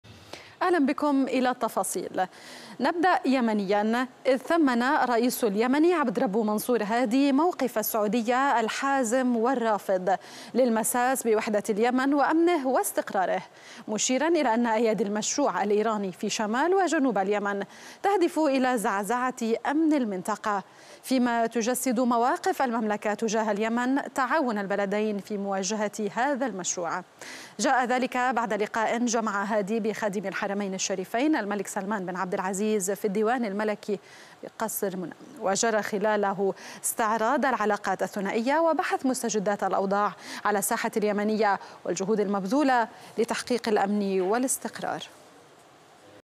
السعودية .. خادم الحرمين يلتقي الرئيس اليمني عبدربه منصور هادي
أخبار MBC

مقدمي النشرة :

حمود_الفايز, غوى_إبراهيم السعودية ..
خادم الحرمين يلتقي الرئيس اليمني عبدربه منصور هادي